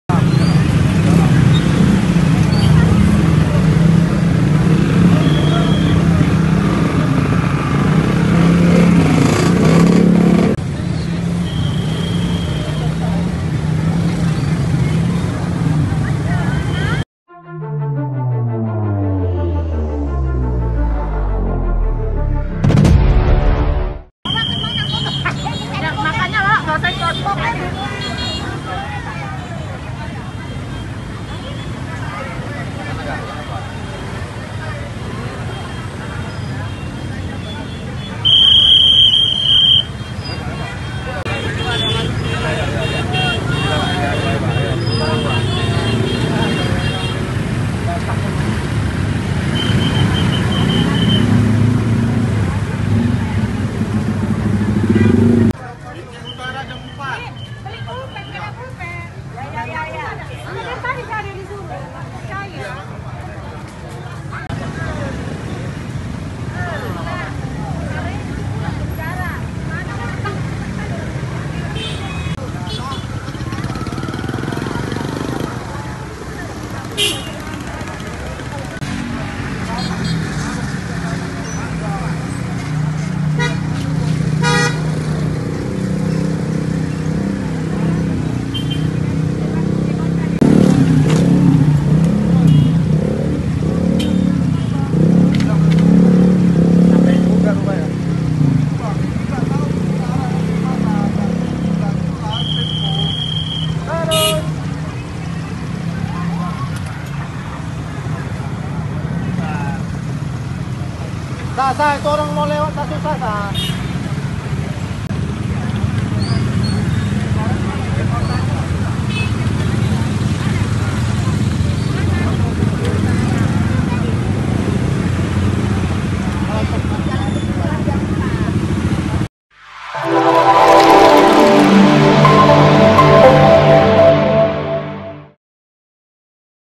Kerumunan Tak Terelakkan, Ratusan Warga Padati Kantor Dinsos Untuk Urus Bantuan Tunai
TRIBUN-MEDAN.COM, BINJAI - Kerumunan orang terlihat memadati Dinas Kota Binjai, di Jalan Gatot Subroto, Kecamatan Binjai Barat, Jumat (14/1/2022).

Diperkirakan raturan orang datang memadati kantor tersebut, guna mengurus bantuan tunai dari Pemerintah.

Bahkan, kemacetan panjang tak terelakan, akibat kerumunan orang yang mengurus bantuan tunai tersebut. Petugas Lantas Polres Binjai langsung turun ke lokasi, guna mengurai kemacetan yang terjadi.